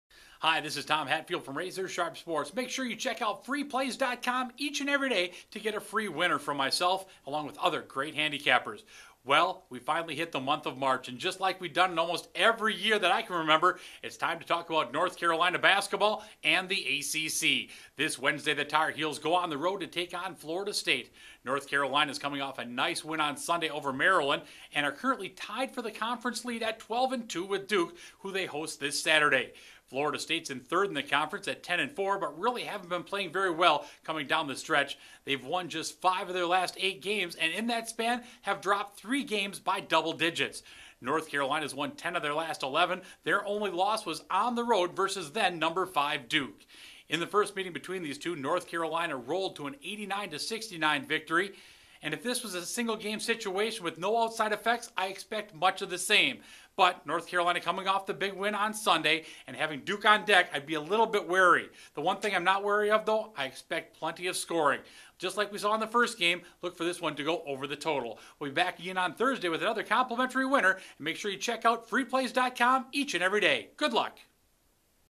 Free Pick 15: NCAA North Carolina vs Florida State, 3/2/2011
Wednesday, March 2, 2011, 7:00 PM Est
721 North Carolina
722 Florida State

To get the most up-to-date odds and scores on this game, make sure

For more information from Tom Hatfield and Razor Sharp Sports,